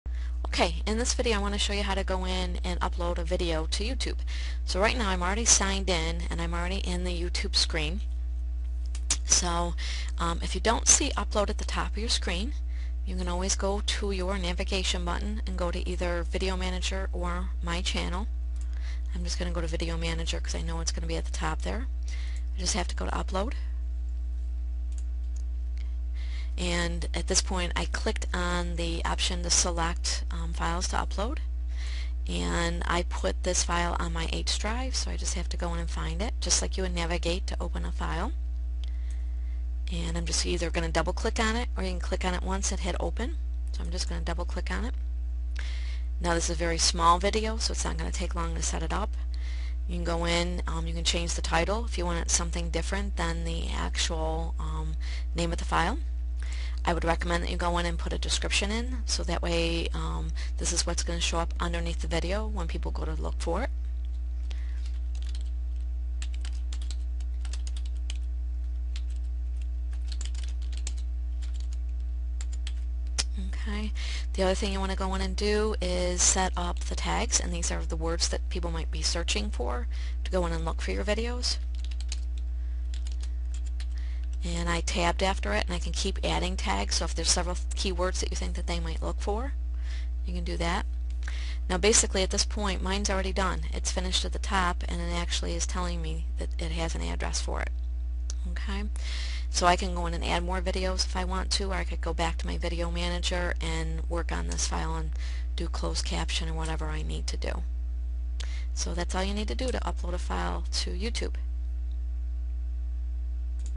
How To Upload a File to YouTube
Instructions on how to upload a file to YouTube.